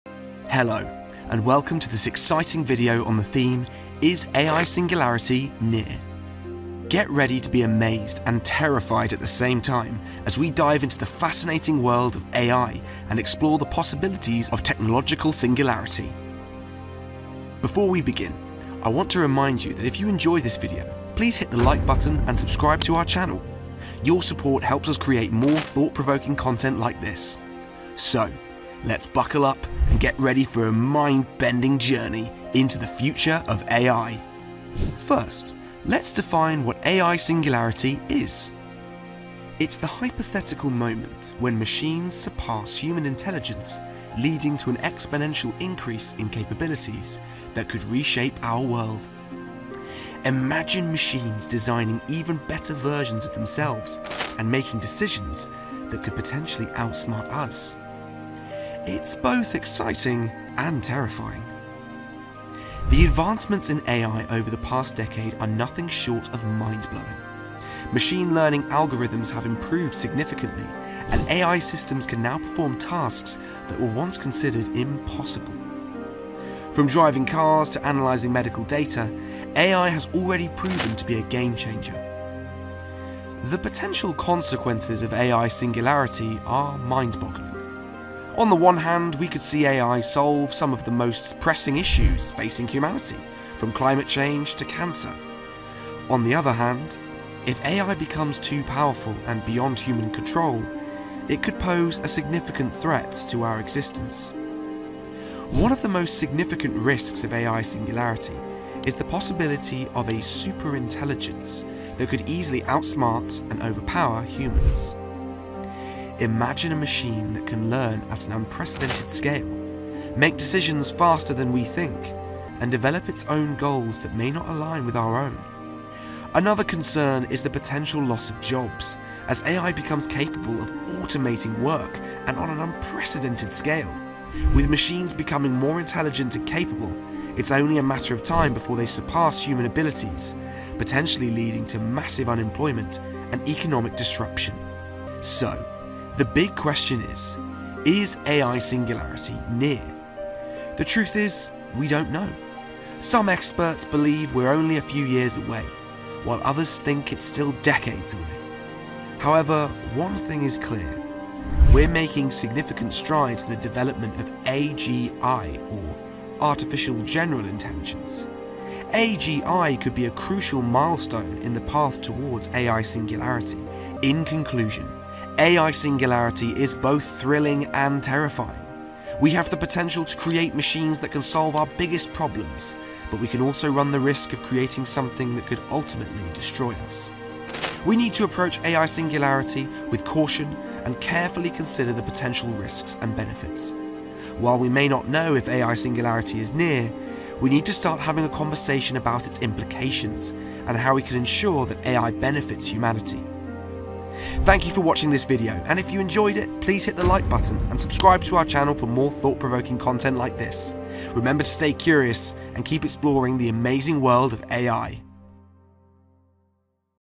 AI Singularity is Near! (ChatGPT-4 New Release!)
With the new release of ChatGPT, ChatGPT-4, AI Singularity is closer than ever before. However, is it an immediate threat? In this video we will investigate the meaning by AI Singularity, and how near it is.

The discussion is done with the help of OpenAI, and the discussion touches whether AI Singularity is reached by the use of AGI, Artificial General Intellingence.

As always we appreciate if you comment a topic you would like us to investigate further.